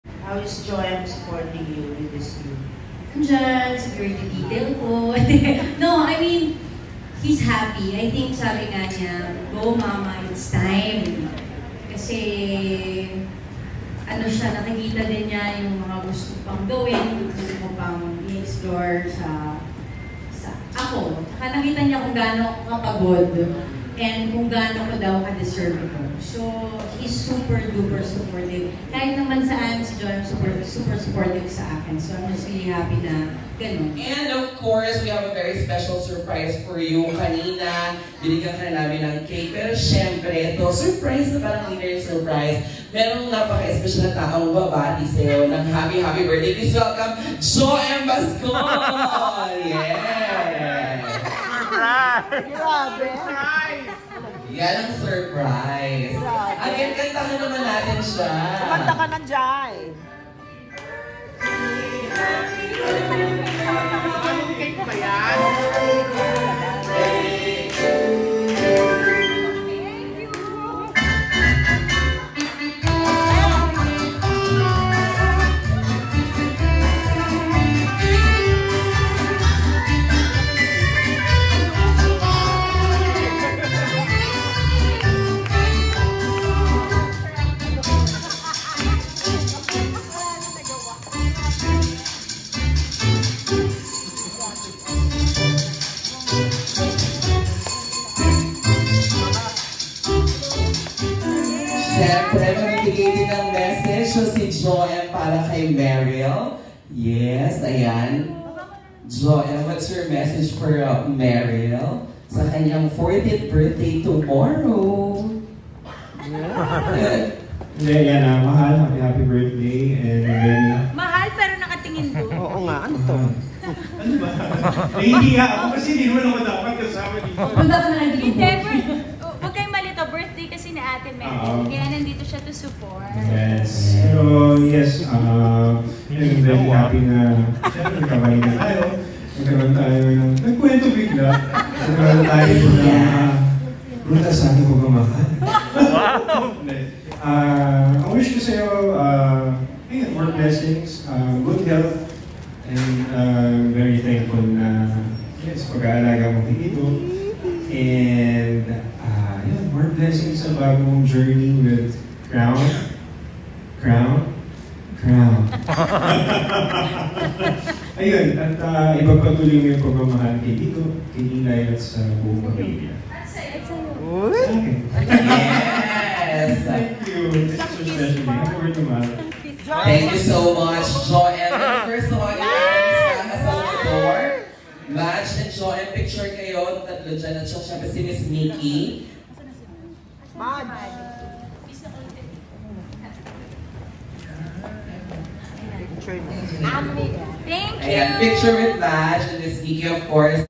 JOEM Surprises MERYLL with a BIRTHDAY Cake on HER 40th Birthday! HE Gets a KISS From the GIRLFIE!
Crown Artist Management (CAM) officially welcomes Ms. Meryll Soriano as their newest contract artist via an intimate presscon held at Studio 28 Lounge in Uptown Parade, BGC. It also coincides with Meryll's 40th Birthday.